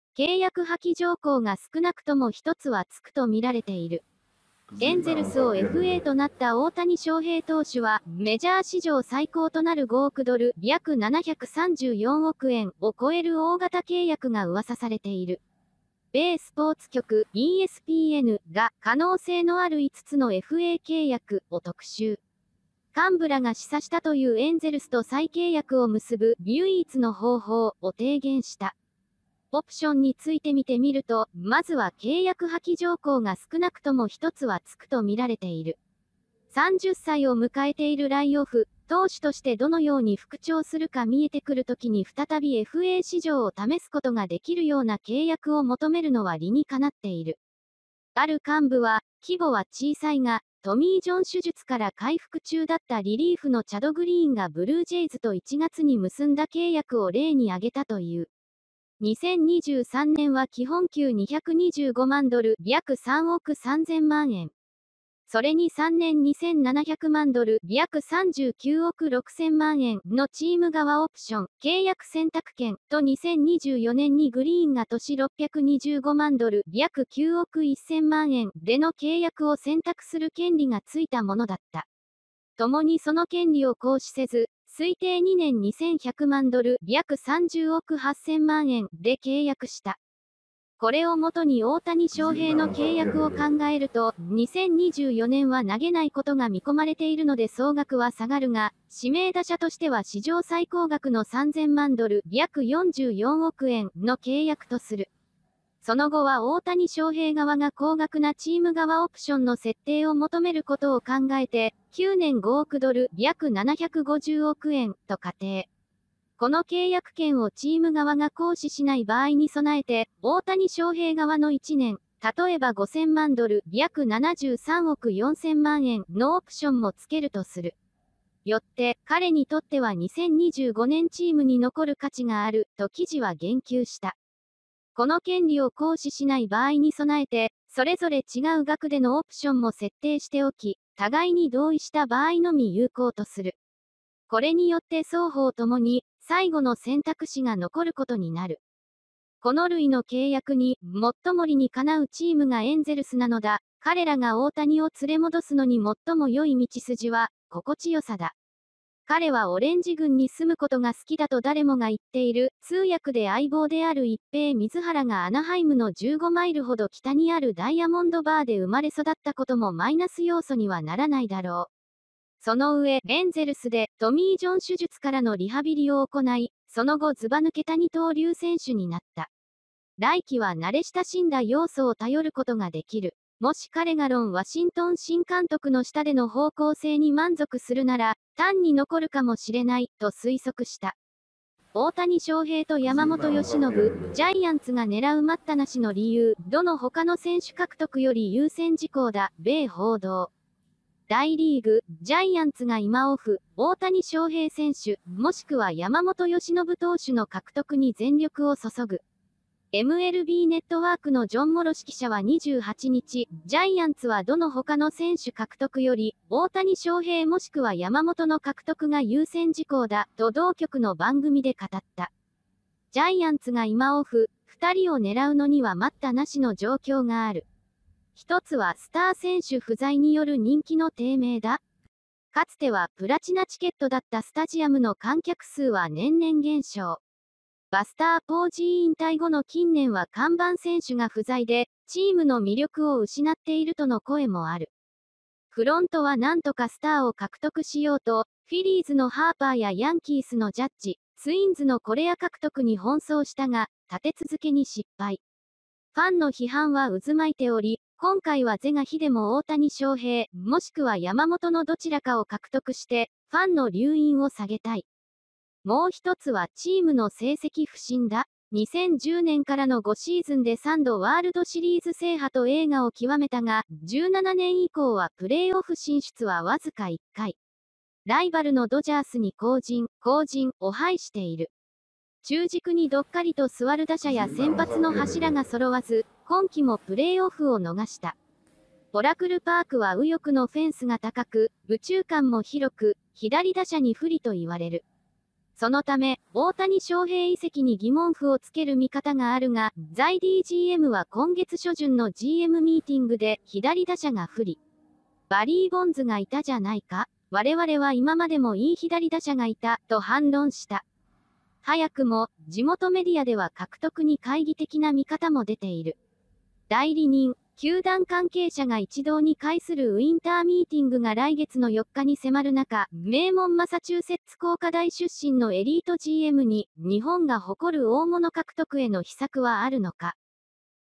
エ軍が大谷翔平を引き留めるには 750億円競争も…利害一致の「最後の選択肢」を米指摘
契約破棄条項が少なくとも1つは付くとみられている

　エンゼルスをFAとなった大谷翔平投手は、メジャー史上最高となる5億ドル（約734億円）を超える大型契約が噂されている。米スポーツ局「ESPN」が「可能性のある5つのFA契約」を特集。幹部らが示唆したというエンゼルスと再契約を結ぶ“唯一の方法”を提言した。

　オプションについてみてみると、まずは契約破棄条項が少なくとも1つは付くとみられている。30歳を迎えている来オフ、投手としてどのように復調するか見えてくる時に再びFA市場を試すことができるような契約を求めるのは理に叶っている。

　ある幹部は、規模は小さいが、トミー・ジョン手術から回復中だったリリーフのチャド・グリーンがブルージェイズと1月に結んだ契約を例に挙げたという。2023年は基本給225万ドル(約3億3000万円）。それに3年2700万ドル（約39億6000万円）のチーム側オプション（契約選択権）と2024年にグリーンが年625万ドル（約9億1000万円）での契約を選択する権利がついたものだった。ともにその権利を行使せず、推定2年2100万ドル（約30億8000万円）で契約した。